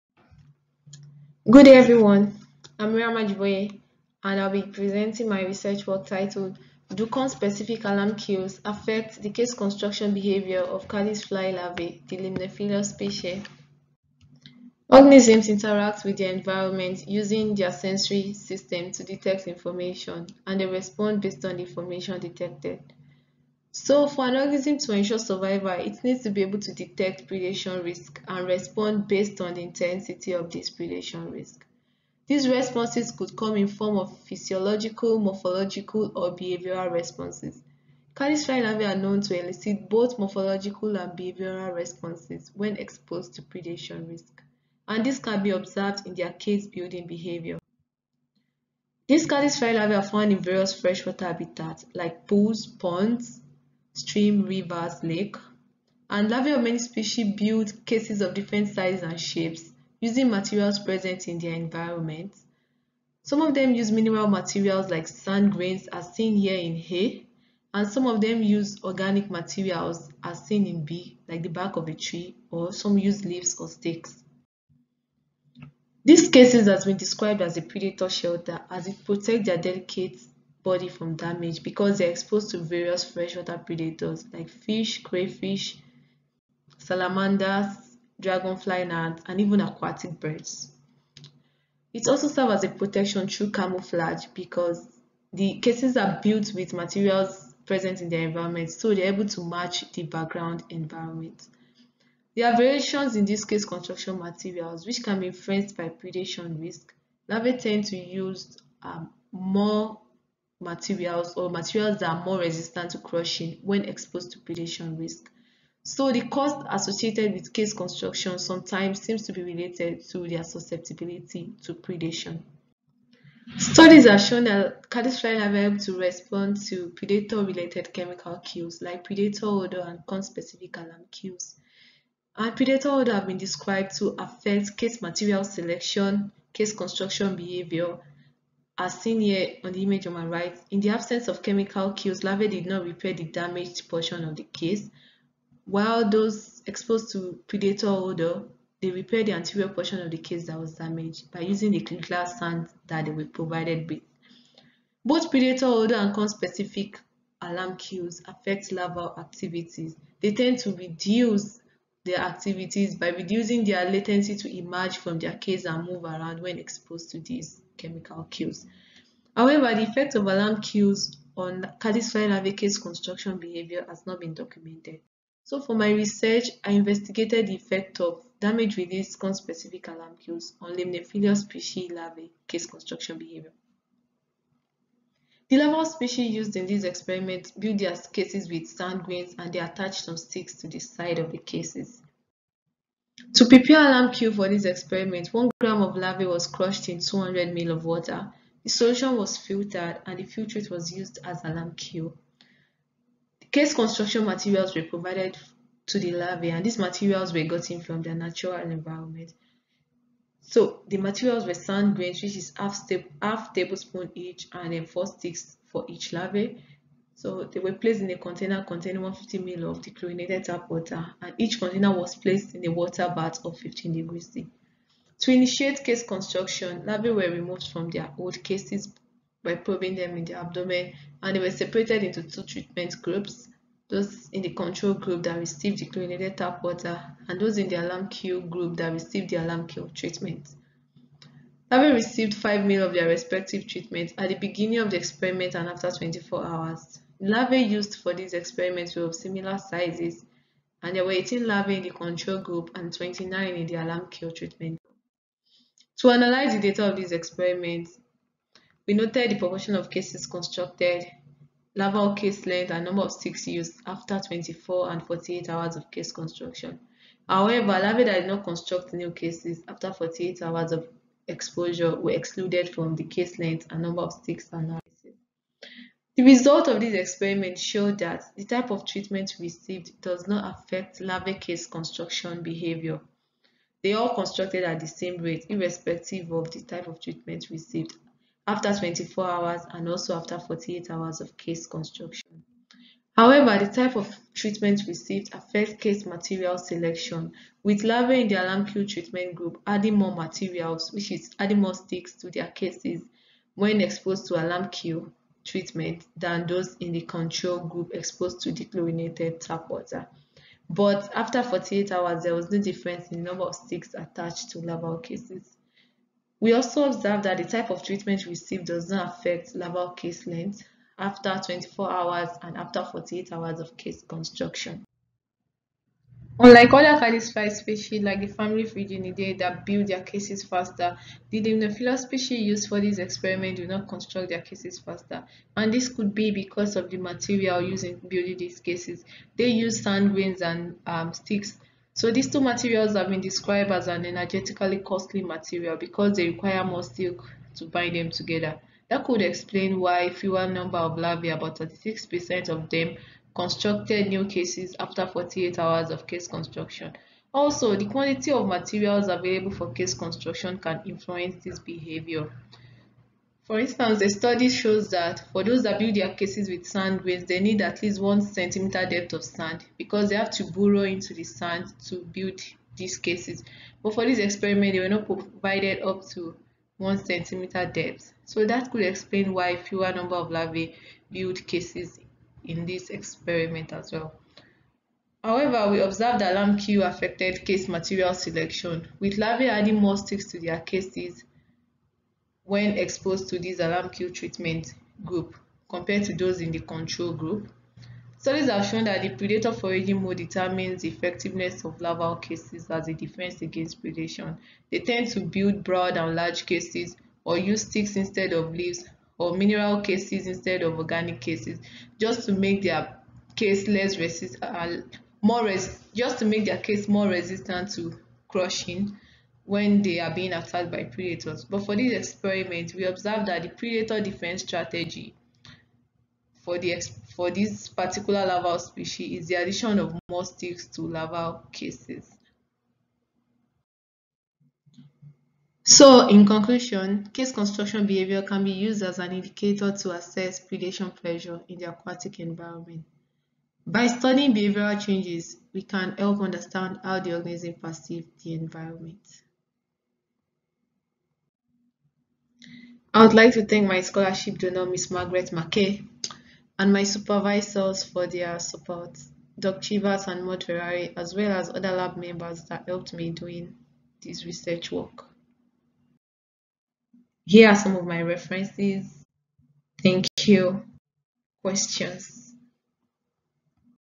DO CONSPECIFIC ALARM CUES AFFECT THE CASE CONSTRUCTION BEHAVIOUR OF... [Ajiboye, Maryam O.]
Maryam O. Ajiboye, Doug P. Chivers, Maud C. O. Ferrari

The portable case of a caddisfly larva is known to have evolved as a defense against predators. Many larval species construct cases of varying sizes and shapes with surrounding materials, and these materials are glued together using silk secreted by the larva. Aquatic prey can use chemical cues from either the predator, damaged conspecifics, or known heterospecifics, to detect predators. Caddisfly larvae inhabit most freshwater habitats, and it has been documented that they are able produce and perceive chemical cues that affect their case construction behaviour. We examined Limnephilus spp. larvae case construction behaviour in response to conspecific damage-released alarm cues. Exposure to alarm cues had no effect on the proportion of larvae that constructed cases or larval case length when compared to those in the control group. However, larvae in the alarm cue treatment group added more sticks to their cases after 24 hours of exposure than those in the control group. This study indicates that alarm cues affect case construction material selection in this species.